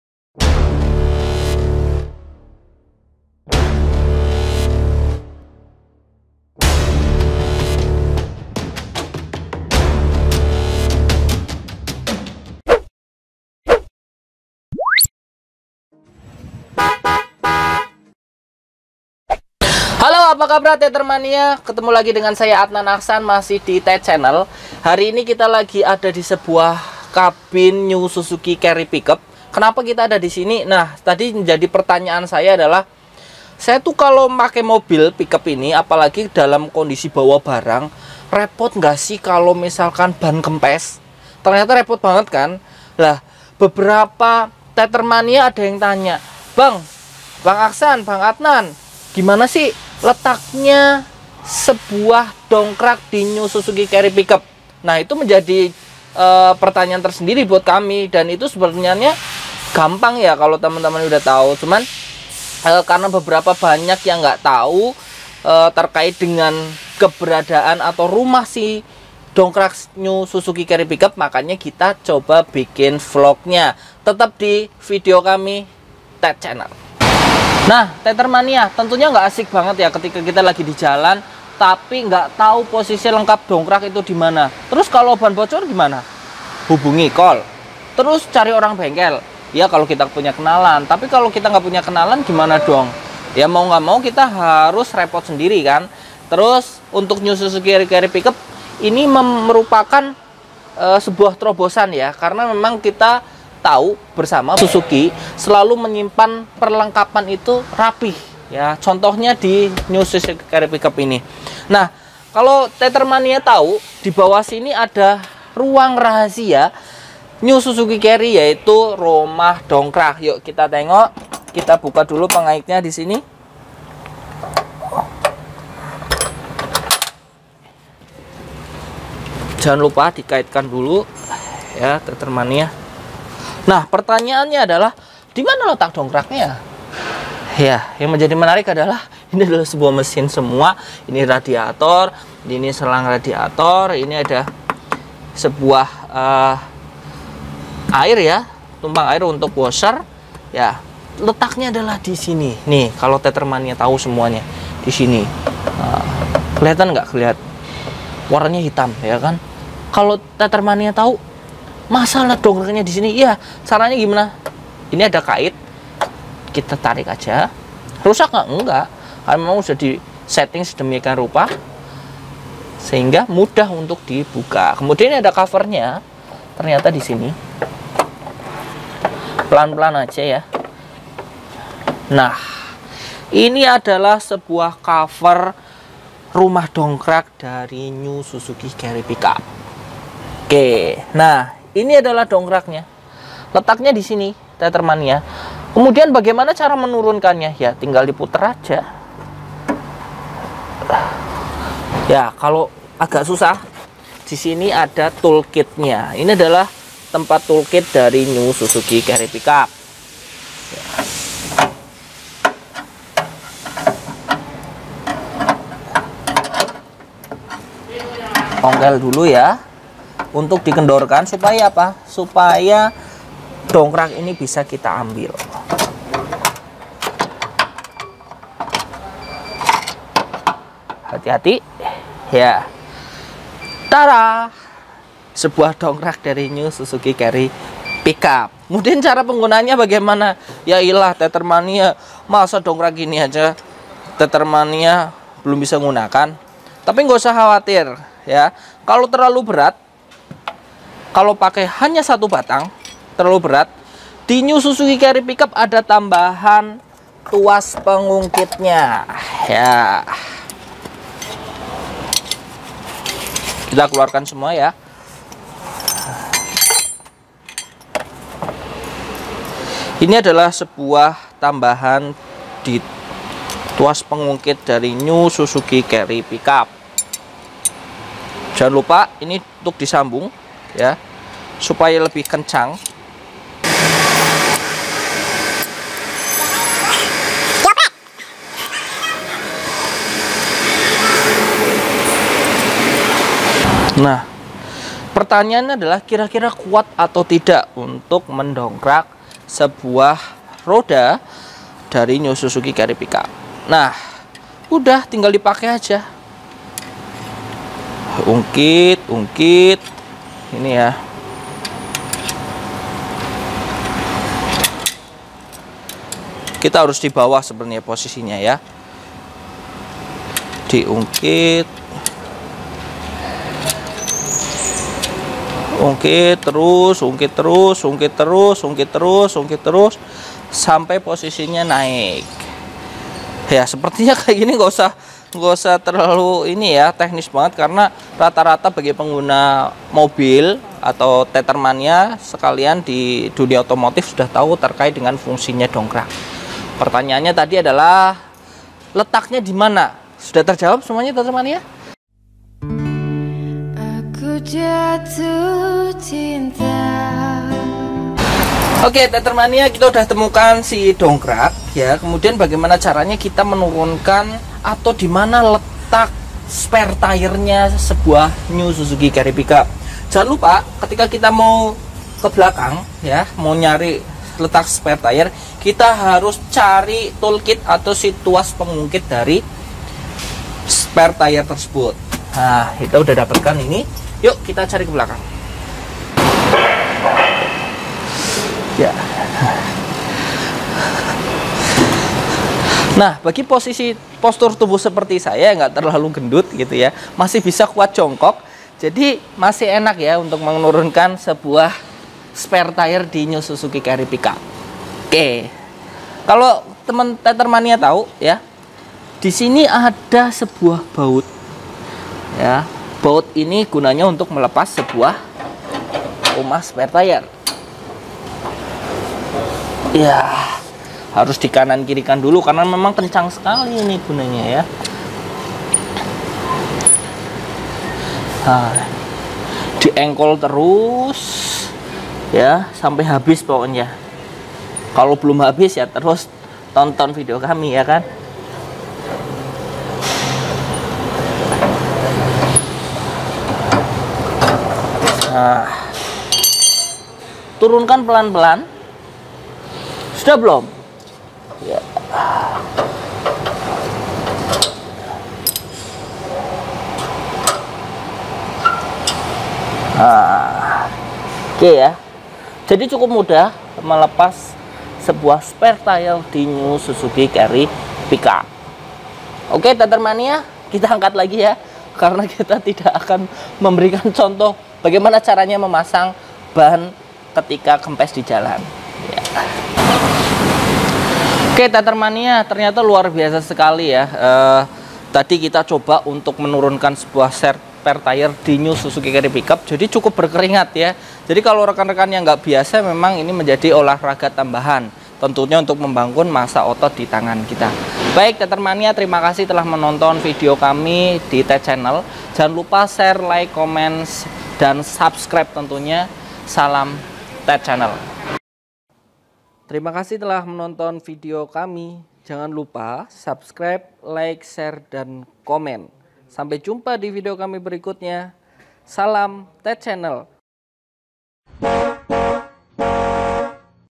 Letak Dongkrak dan Cara Turunin Ban Serep New Carry PU
Prends biar tidak bingung saat kondisi darurat, mungkin ban ada yg bocor dan perlu diganti dengan ban serep, terkadang para pengguna Mobil niaga New Carry PU tidak tahu atau lupa letak dongkrak dimana dan blm tau cara untuk menurunkan ban serep dari lokasi penyimpanannya..Berikut kami share videonya bagi yang belum mengetahui letak dongkrak & cara menurunkan ban serep...

Semoga membantu ya